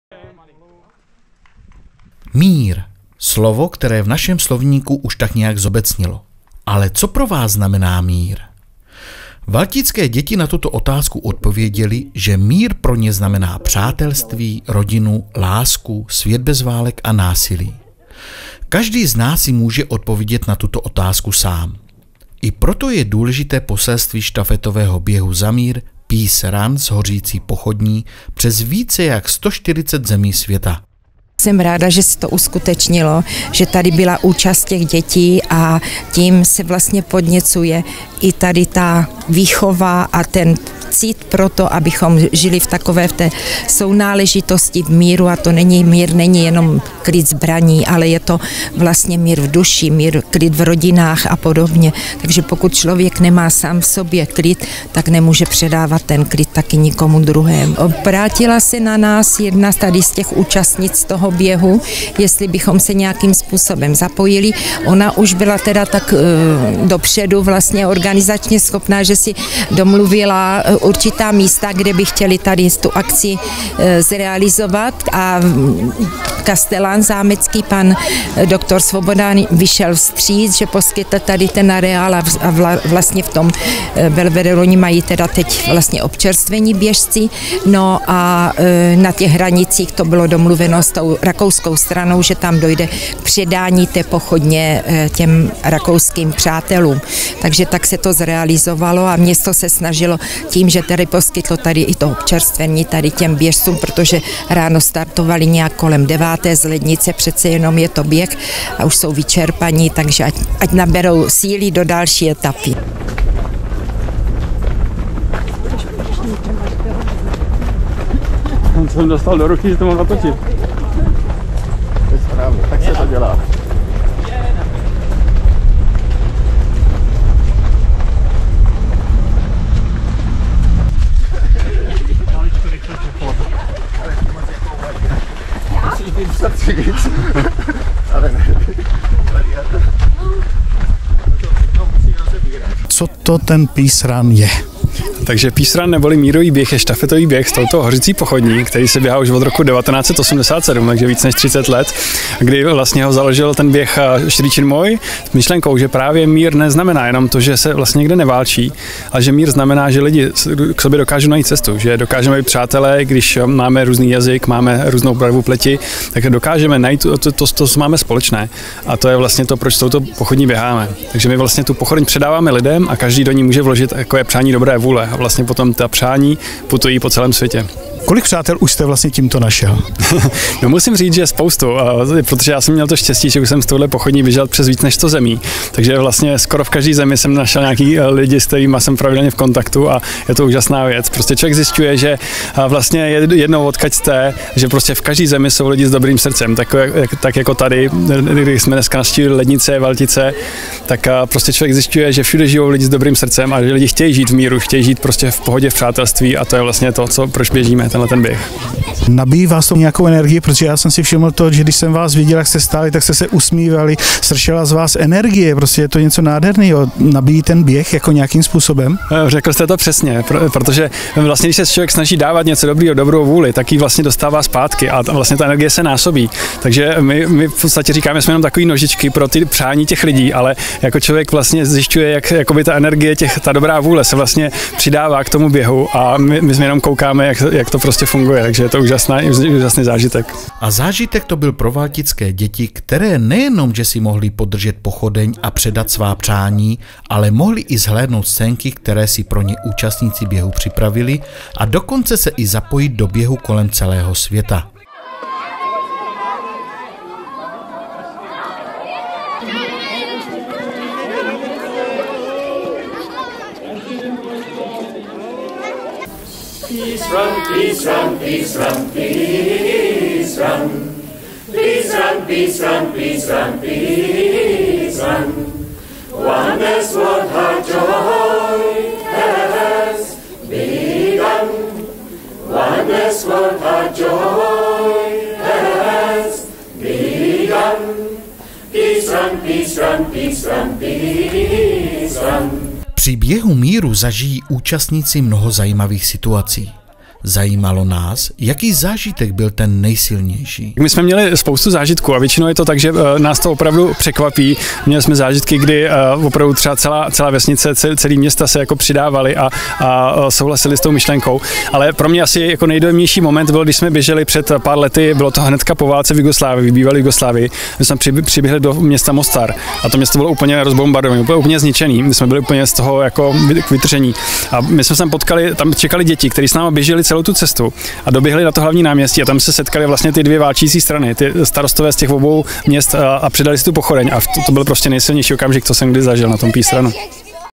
Běh míru, Peace Run zanechal ve Valticích úsměv a radost
Mír, slovo, které v našem slovníku už tak nějak zobecnělo, ale co pro Vás znamená Mír valtické děti na tuto otázku odpověděly, že mír pro ně znamená přátelství, rodinu, lásku svět bez válek a násilí. Každý z nás si může odpovědět na tuto otázku sám. I proto je důležité poselství štafetového běhu za mír Peace Run s hořící pochodní přes více jak 140 zemí světa.